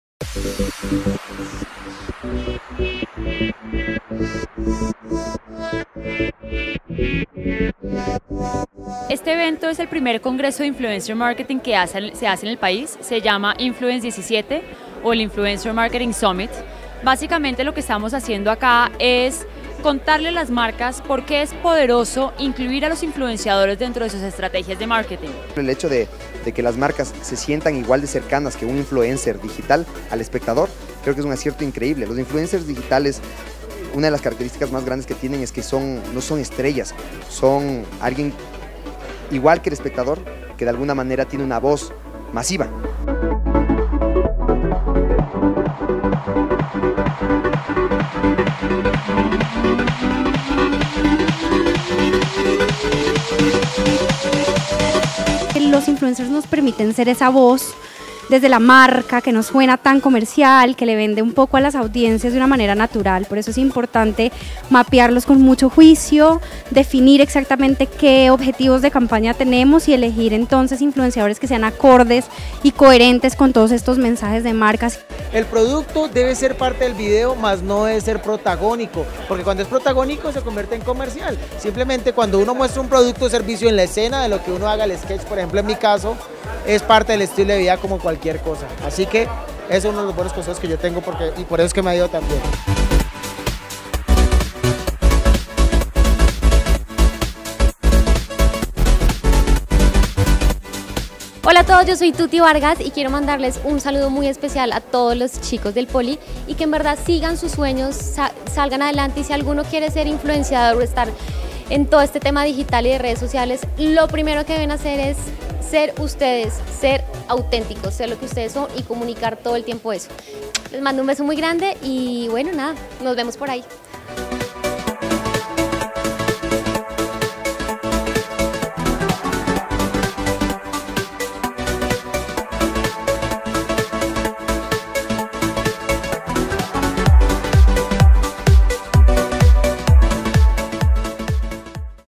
El Poli en el Influencer Marketing Summit 2017
Así vivimos el Influencer Marketing Summir del 2017, un gran evento que reunió a los más importantes influencers para compartir su conocimiento y consejos .